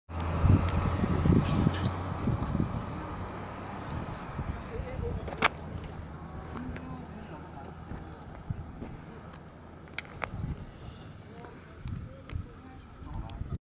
Harley Davidson Tri Glide ultra classic in red rock canyon
Me on Harley Davidson tri glide ultra classic in red rock canyon nevada las vegas. Engine air-cooled OHV V-twin engine (1,690 cc).